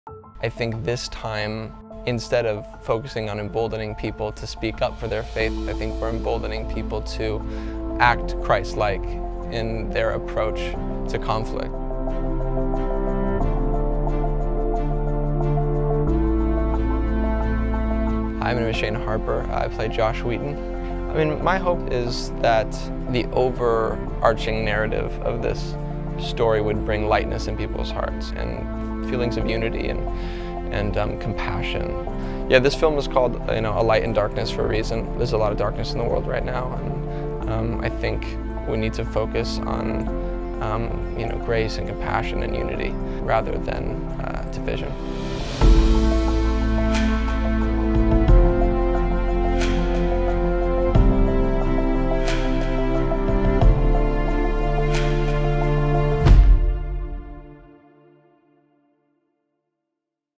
God's Not Dead: A Light in Darkness | Shane Harper
Shane Harper -- back playing the popular character, Josh, from the first film --  talks about the impact of God's Not Dead: A Light in Darkness.

Stream exclusive originals, movies, TV shows, and more on PureFlix.com! Start Your Free Month